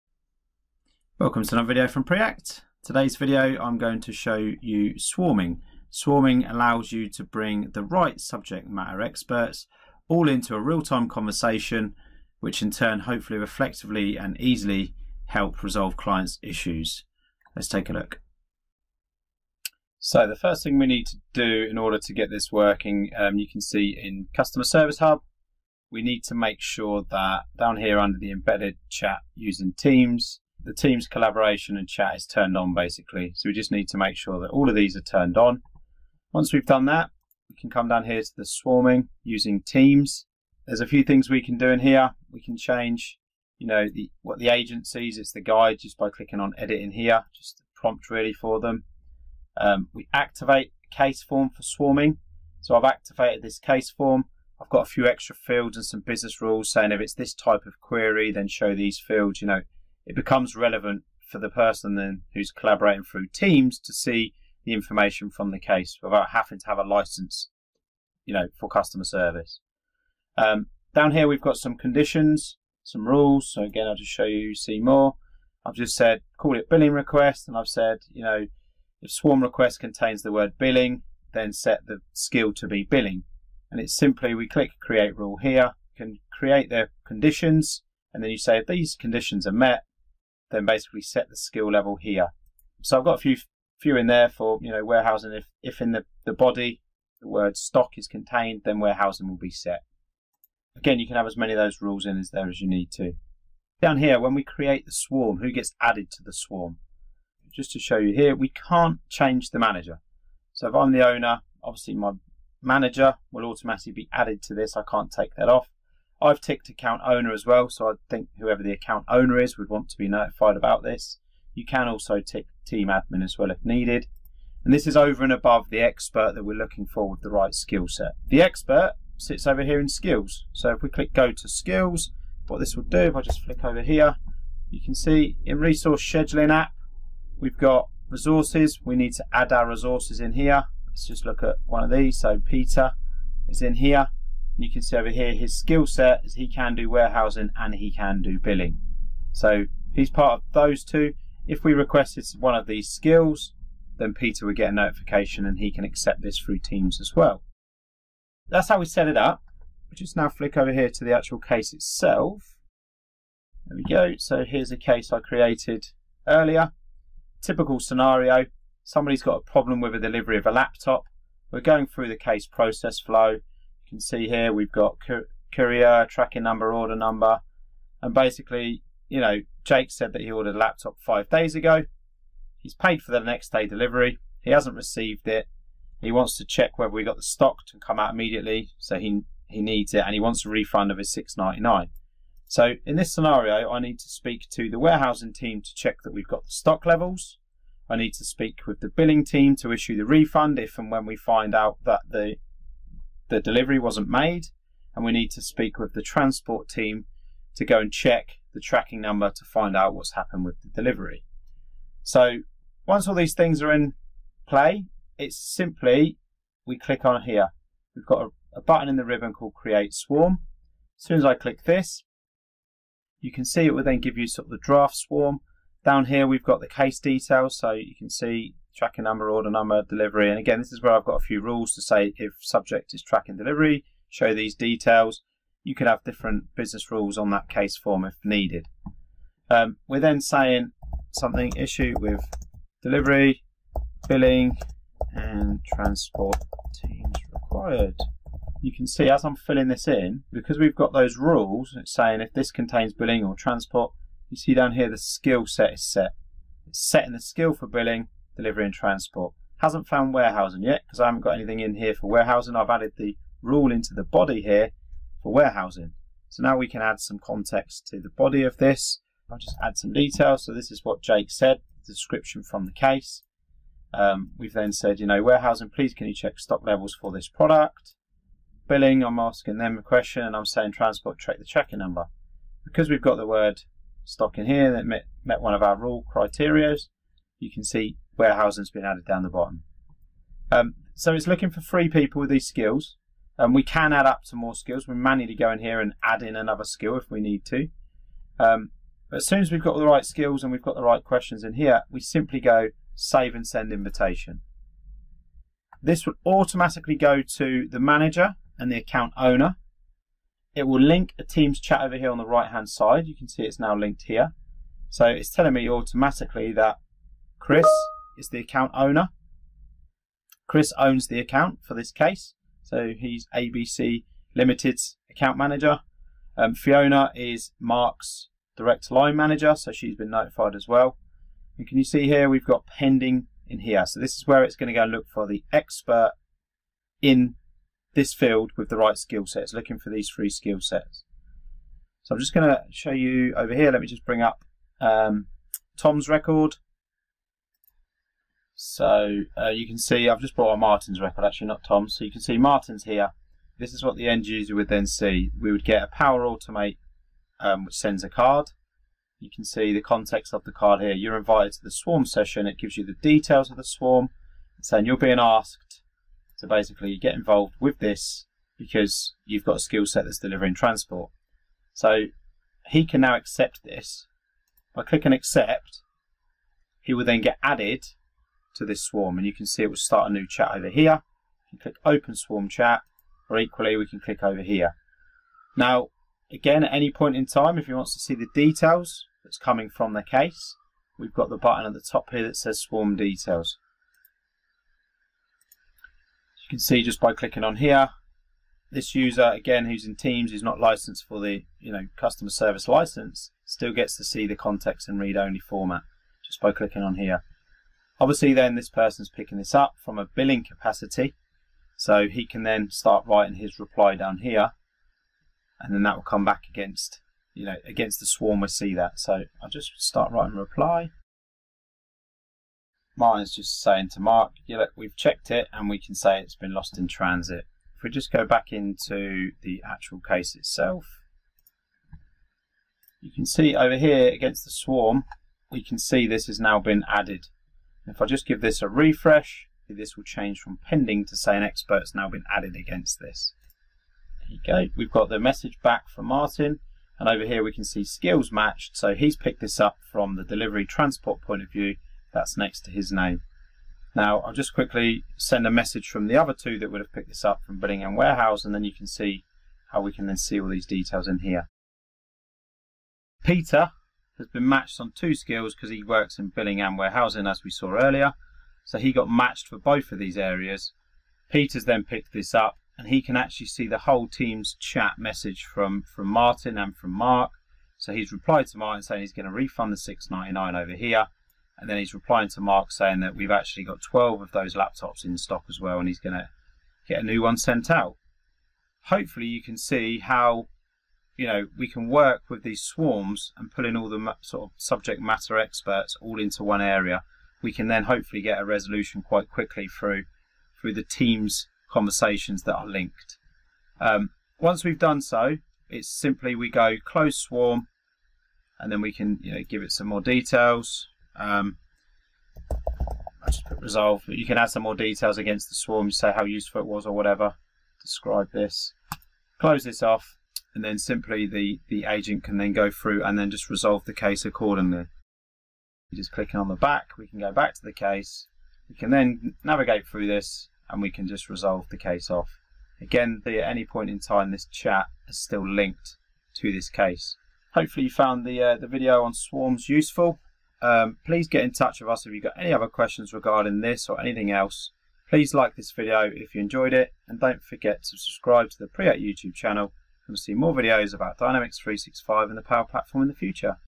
Dynamics 365 Customer Service Support Swarms with Microsoft Teams (Preview)
Demonstrating how Support Swarms help service teams consistently resolve complex customer issues quickly by finding and collaborating with subject matter experts.

In this example, we demonstrate how Support Swarms for Dynamics 365 Customer Service enable agents to identify specialist colleagues who can assist with an individual case using embedded Microsoft Teams chat.

Watch this presentation to see how support swarming allows agents to move more quickly within the familiar Dynamics 365 interface using collaborative Teams capabilities.

Timestamps:
0:00 Introduction
0:19 Enabling embedded Teams chat & collaboration
0:37 Configuring Customer Support Swarming & conditions (Preview)
2:07 Managing subject matter experts
2:42 Support case example involving multiple issues
3:31 Creating a swarm from a case record
4:58 New embedded Team chat created for the swarm on the D365 case
5:49 How subject matter experts accept swarm invites in Teams
6:25 Surfacing D365 case detail in Teams using the swarm chat
6:47 Using the swarm to collaborate and resolve each issue
8:24 Closing the swarm